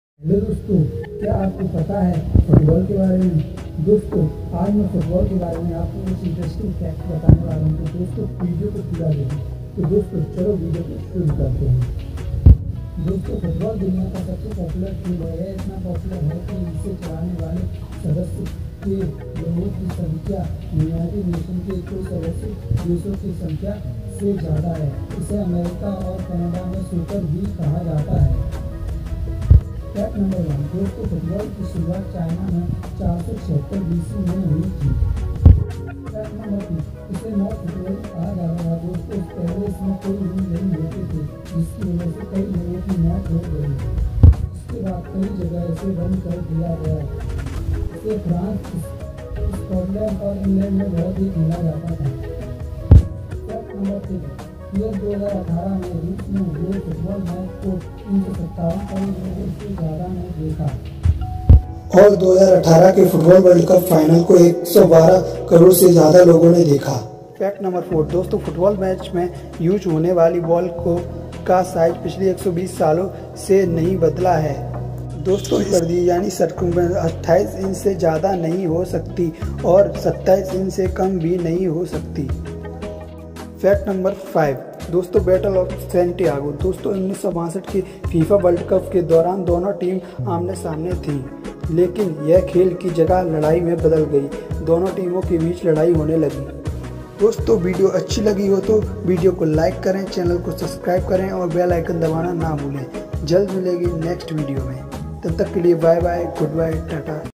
Top 5 fact about football (mk Gyan beast )
hello friends welcome to my channel mk Gyan beast dosto ajj ki is vedio me apko football ke kuchh rochak fact vatane Bala hu to dosto vedio ko Pura dekhe jald melege next vedio me